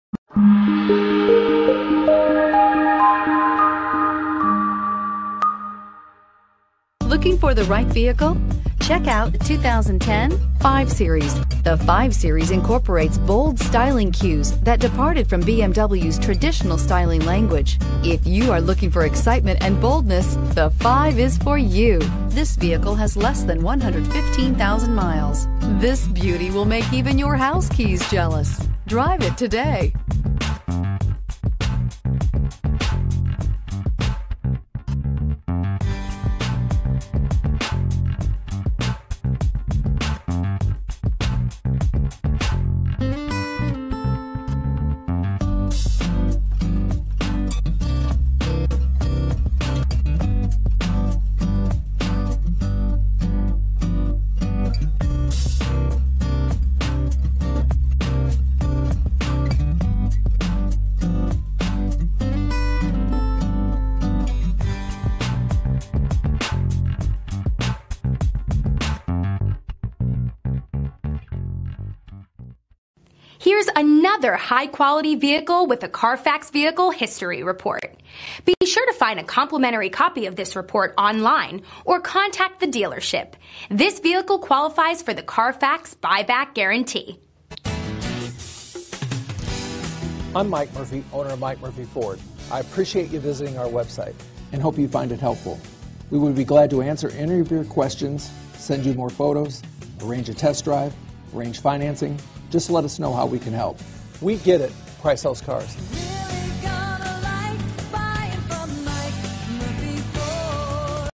2010 BMW 5 Series 2010 BMW 5 Series FOR SALE 389434
2010 BMW 5 Series 4dr Sdn 528i xDrive AWD

565 W. Jackson St.

We get it... Price Sells Cars!! * 6 Cylinder engine *   * Check out this 2010 BMW 5 Series 4dr Sdn 528i xDrive AWD * * 2010 ** BMW * * 5 Series * * 4dr Sdn 528i xDrive AWD *  This Black 2010 BMW 5 Series 4dr Sdn 528i xDrive AWD might be just the sedan awd for you.  It comes with a 6 Cylinder engine.  For a good-looking vehicle from the inside out, this car features a sleek black exterior along with a cream beige interior.  This one scored a crash test rating of 4 out of 5 stars.